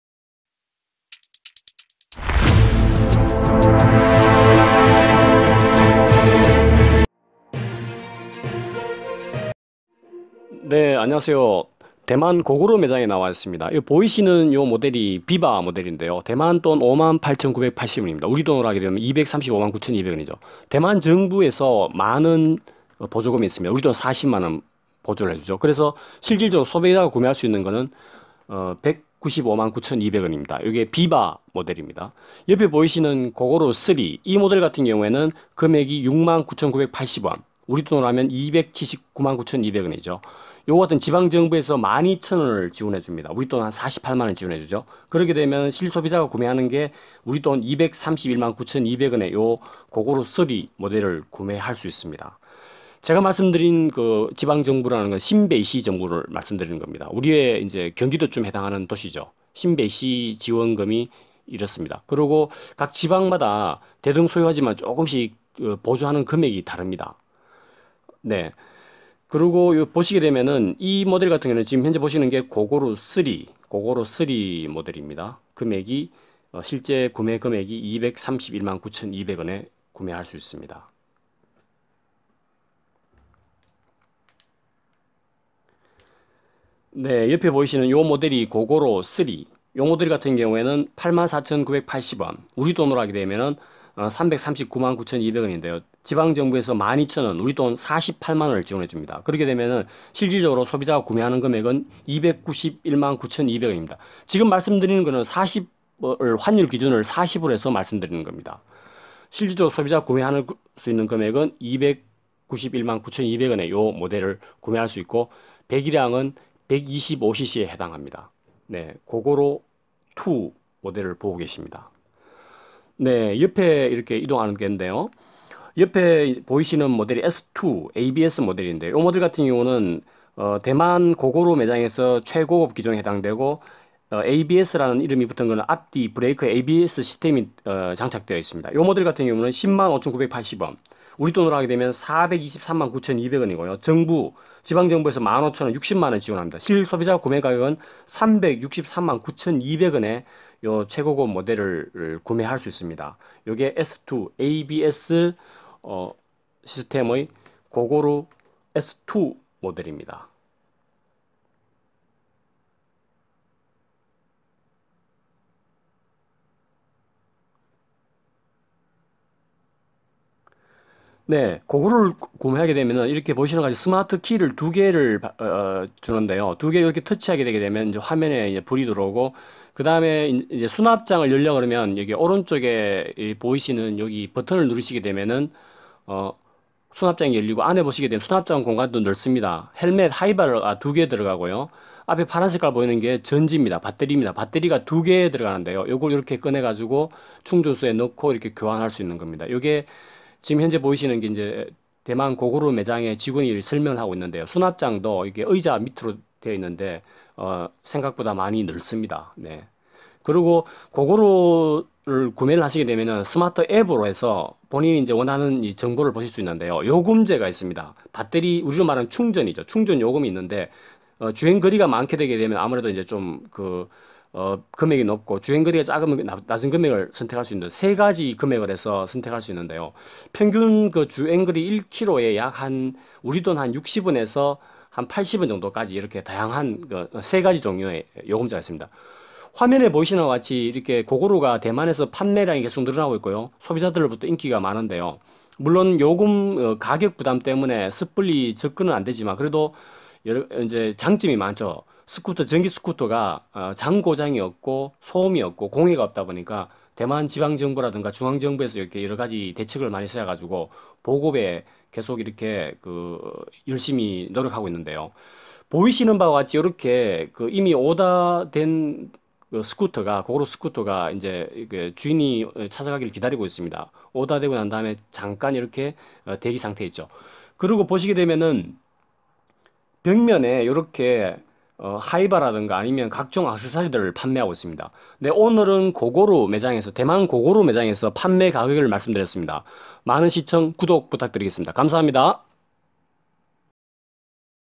고고로 스쿠터 대만 판매가격
고고로 전기 스쿠터 대만 판매가격을 알아 보았습니다. 대만의 고고로 매장에서 판매하고 있는 각 모델의 가격과 지방정부 보조금등을 소개합니다.
스쿠터 생산 대국인 대만도 기존의 기름 연료 스쿠터에서 전기 스쿠터로 대체 하려는 정부의 노력과 각 업체의 노력의 결과로 대만내에서 점점 인기를 더해가고 있지만 가격 적인 부담이 있어 그런지  빠른 성장은 힘들어 보입니다.